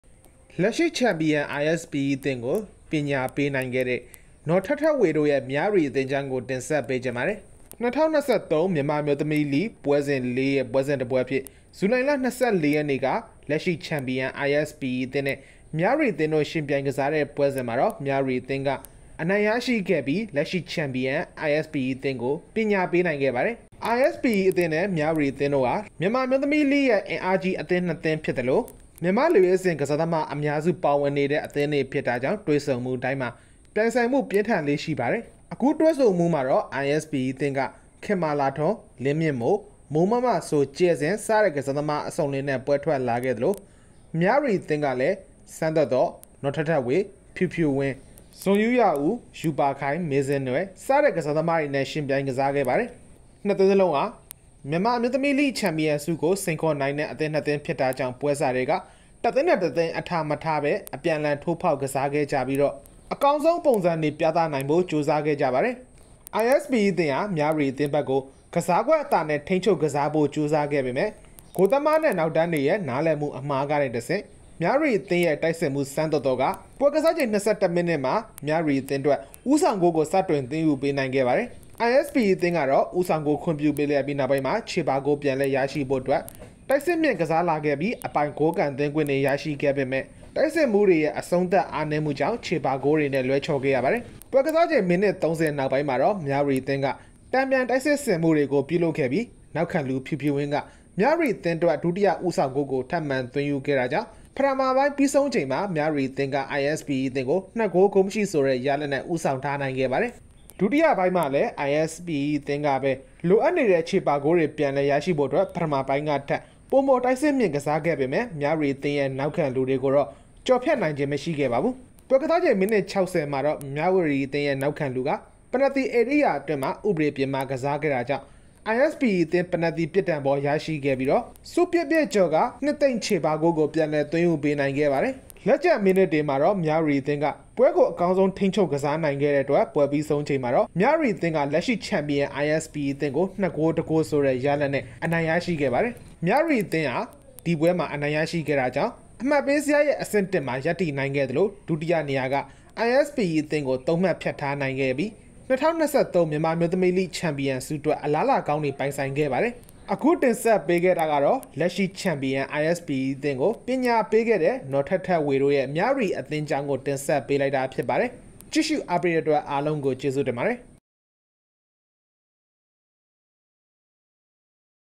Myawadi Women's FC defeated the Myanmar women's league champions ISPE FC.
Myanmar Women's League's current champion ISPE FC was able to beat Myawadi FC.